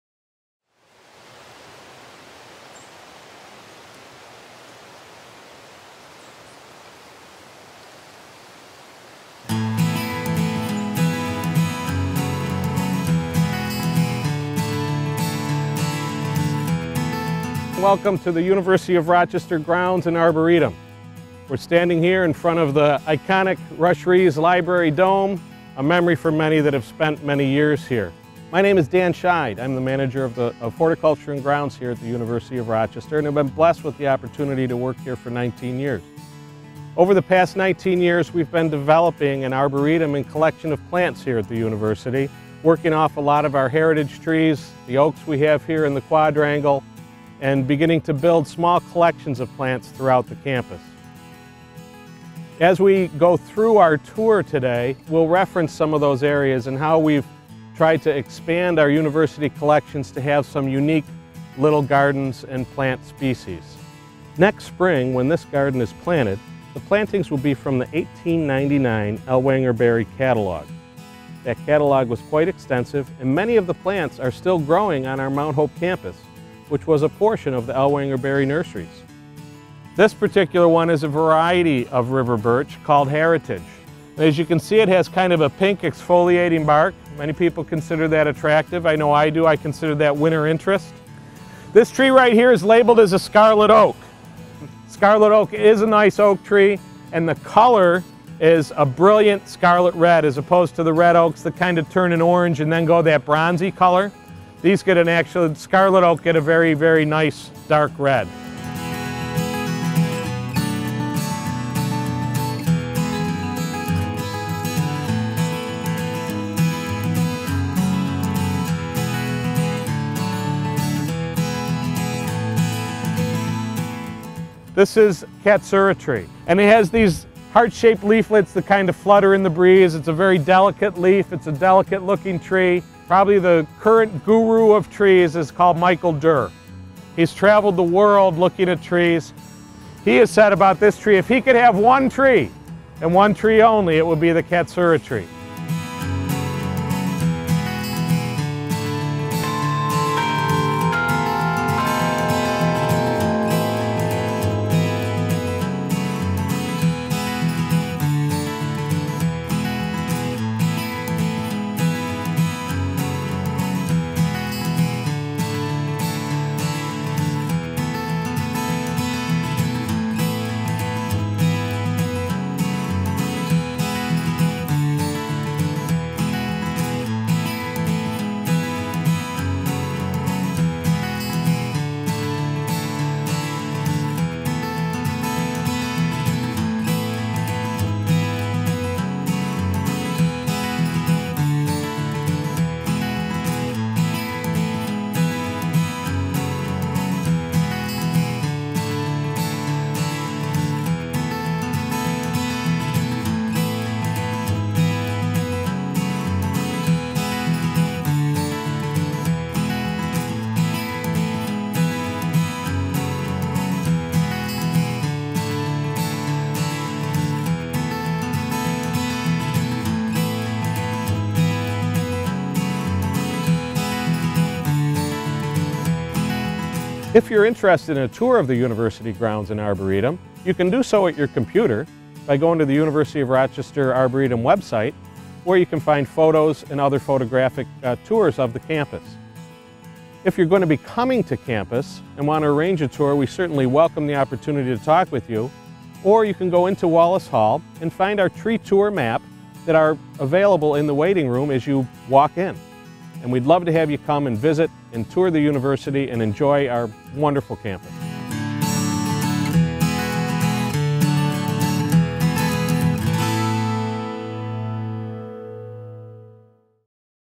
The University of Rochester's River Campus Arboretum
A brief tour of the University grounds.